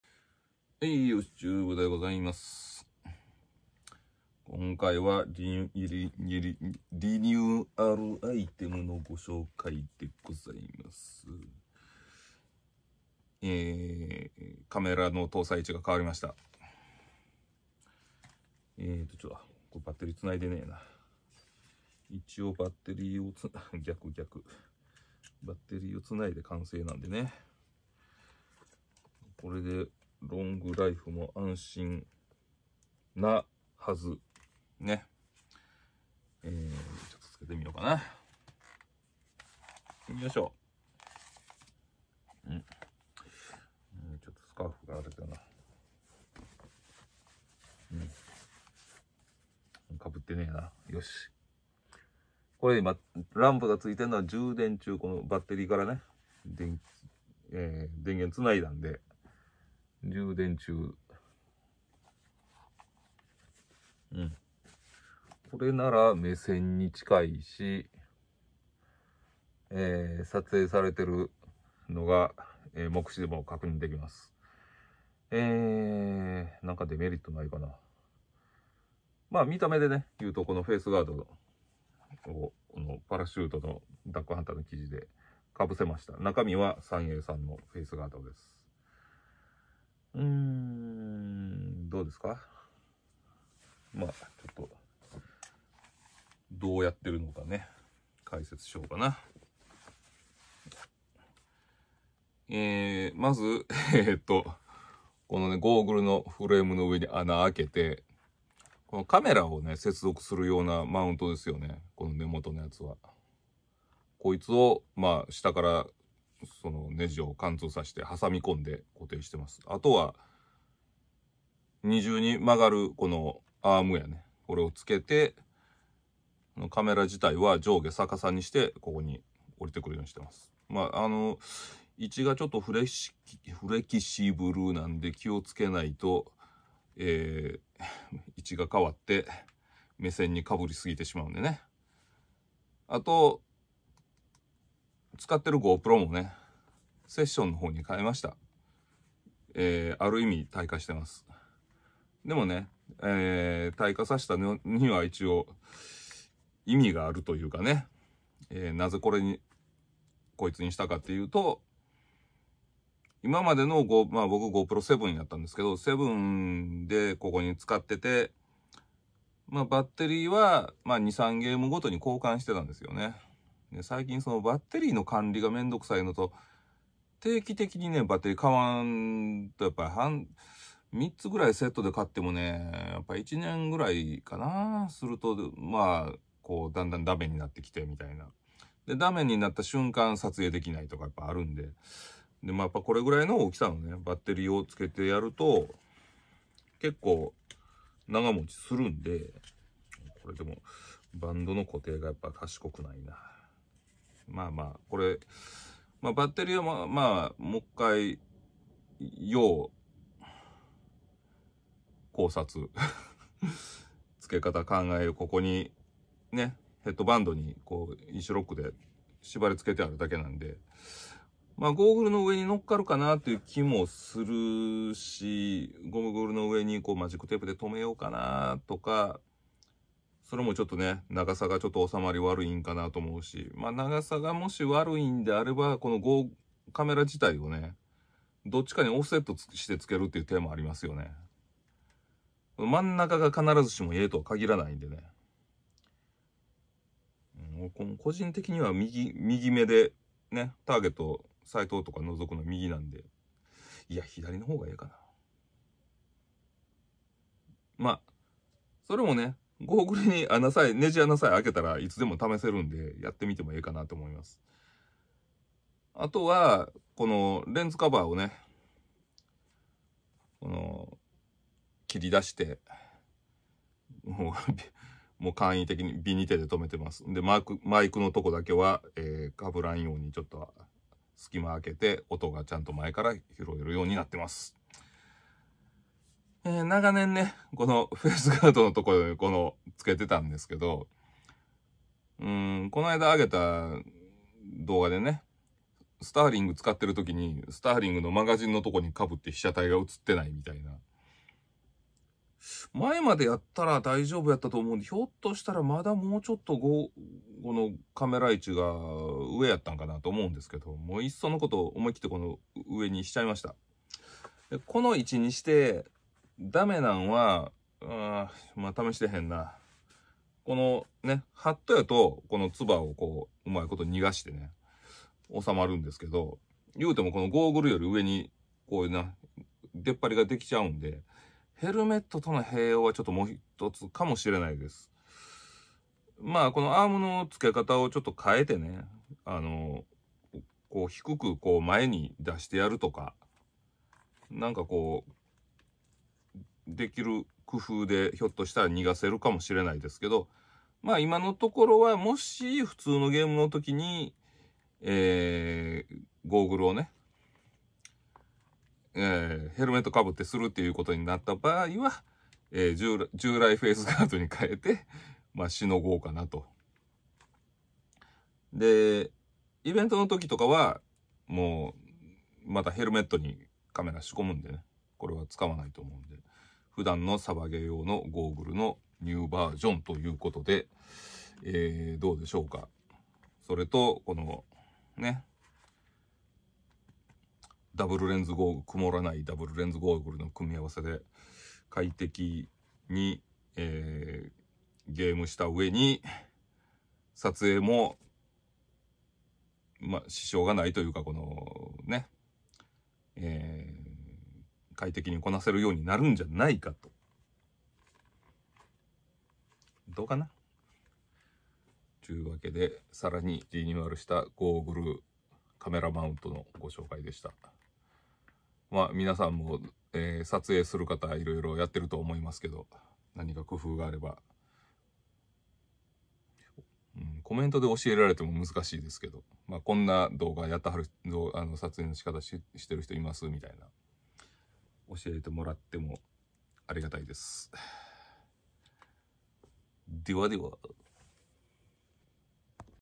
オシ部屋463カメラの新しいレイアウト 2024年3月31日
今までフェイスガードに着けていたカメラをゴーグルにつけてみました